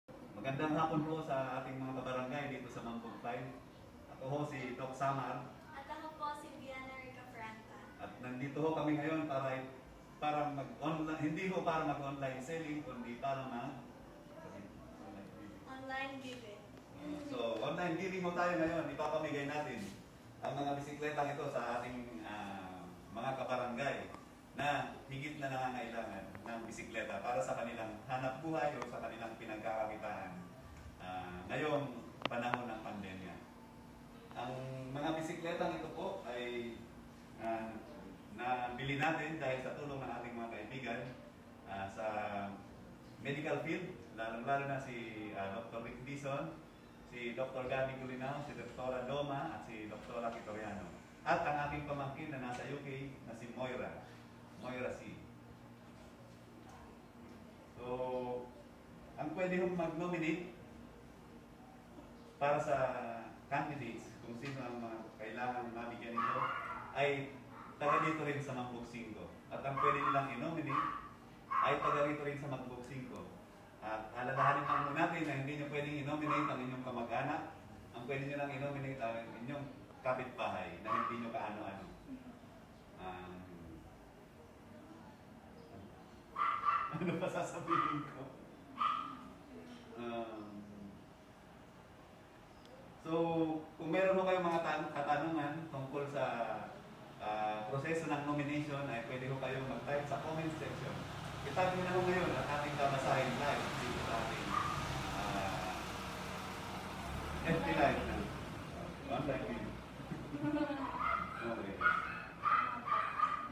BisikletaNgPagasa
sasagutin ko po ang inyong mga katanungan tungkol sa ipinamimigay na bisikleta